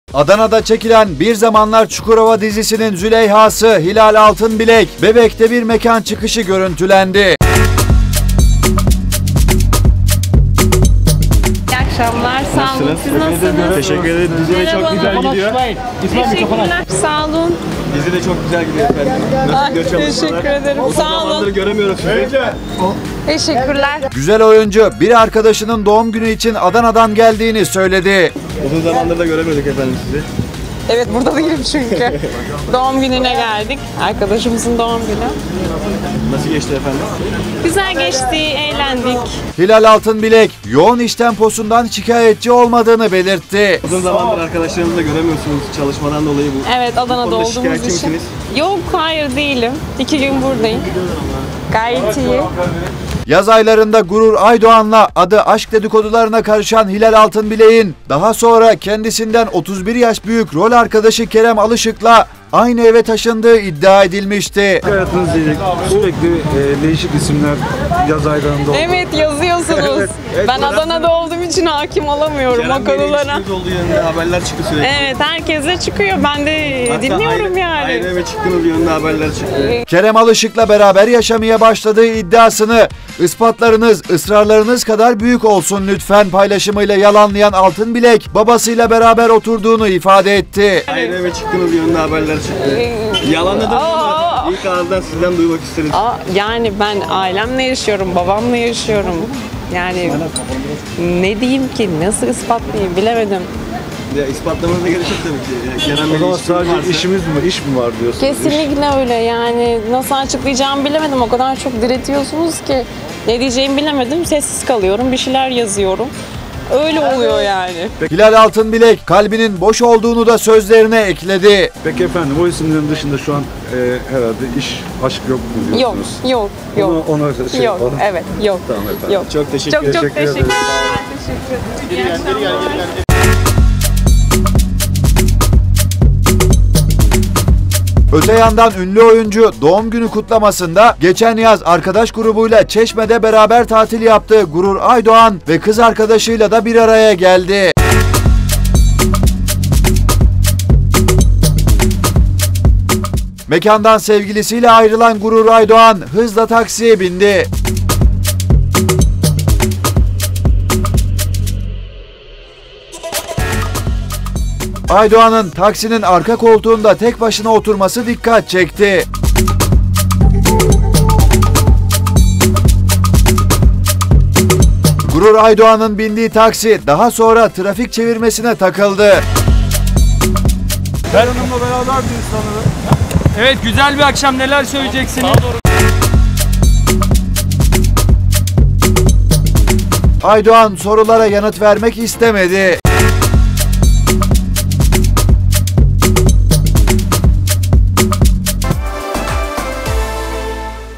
Hilal Altınbilek: Kerem Alışık'la değil, babamla aynı evde yaşıyorum
“Bir Zamanlar Çukurova” dizisinin Züleyha’sı Hilal Altınbilek, rol arkadaşı Kerem Alışık’la aynı evde yaşamaya başladığı iddiası hakkında neler söyledi?

Adana’da çekilen “Bir Zamanlar Çukurova” dizisinin Züleyha’sı Hilal Altınbilek, Bebek’te bir mekân çıkışı görüntülendi. Güzel oyuncu bir arkadaşının doğum günü için Adana’dan geldiğini söyledi. Hilal Altınbilek, yoğun iş temposundan şikayetçi olmadığını belirtti.

Yaz aylarında Gurur Aydoğan’la adı aşk dedikodularına karışan Hilal Altınbilek’in daha sonra kendisinden 31 yaş büyük rol arkadaşı Kerem Alışık’la aynı eve taşındığı iddia edilmişti.

Kerem Alışık’la beraber yaşamaya başladığı iddiasını “İspatlarınız ısrarlarınız kadar büyük olsun lütfen” paylaşımıyla yalanlayan Altınbilek, babasıyla beraber oturduğunu ifade etti. Hilal Altınbilek, kalbinin boş olduğunu da sözlerine ekledi.

Öte yandan ünlü oyuncu doğum günü kutlamasında, geçen yaz arkadaş grubuyla Çeşme’de beraber tatil yaptığı Gurur Aydoğan ve kız arkadaşıyla da bir araya geldi.

Mekândan sevgilisiyle ayrılan Gurur Aydoğan hızla taksiye bindi. Aydoğan’ın taksinin arka koltuğunda tek başına oturması dikkat çekti. Gurur Aydoğan’ın bindiği taksi daha sonra trafik çevirmesine takıldı. Aydoğan, sorulara yanıt vermek istemedi.

tv100 uydu bilgileri:

TÜRKSAT 4A
Frekans: 12729
Sembol: 30000
Polarizasyon: V (Dikey)